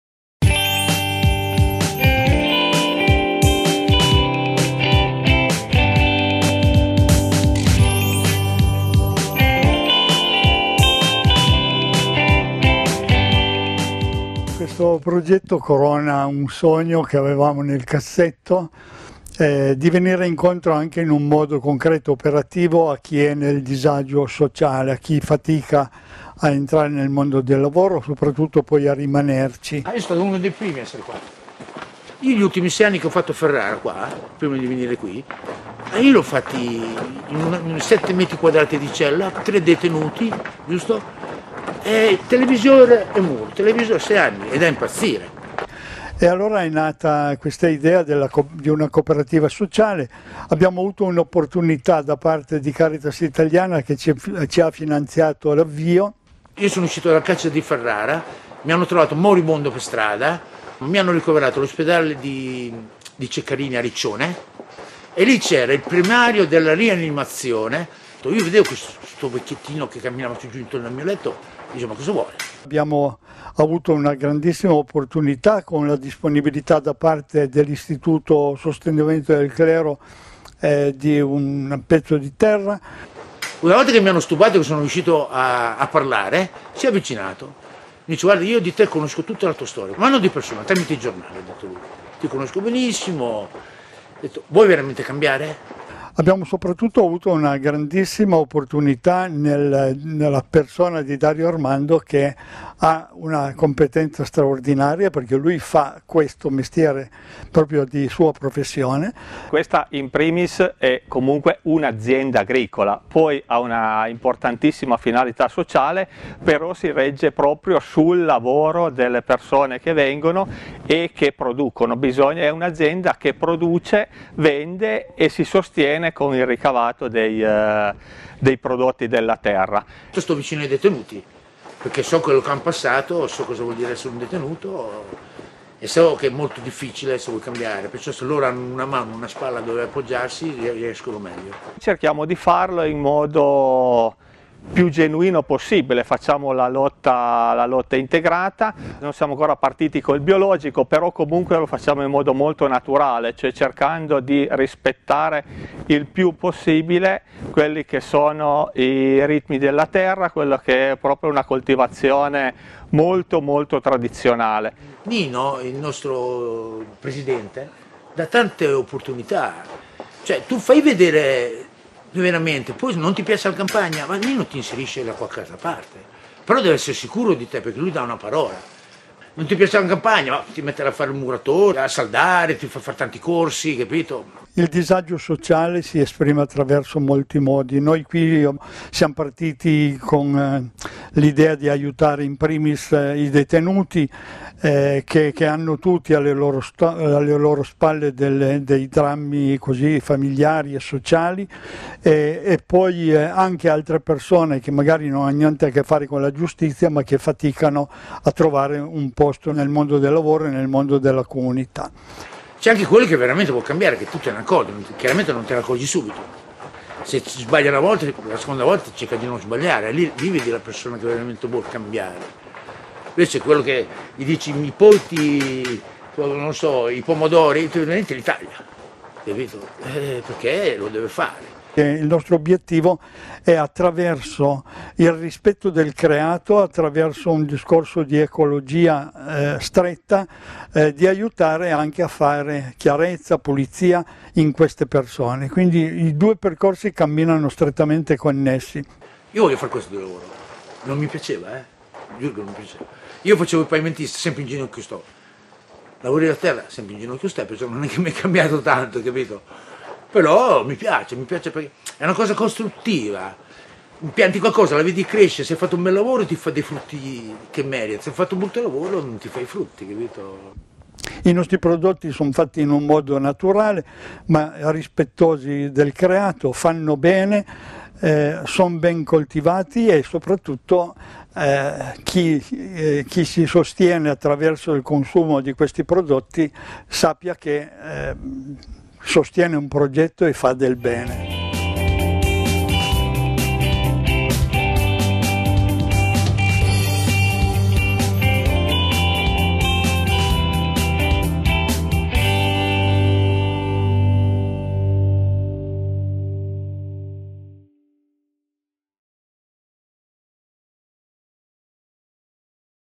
Una serra per cambiare
Il video realizzato dalla Pastorale Sociale e del lavoro in preparazione della Settimana sociale dei Cattolici: l'esperienza di Cascina Pensolato è stata individuata tra le "Buone pratiche della Granda" necessarie per indicare i passi concreti da compiere verso un'ecologia integrale.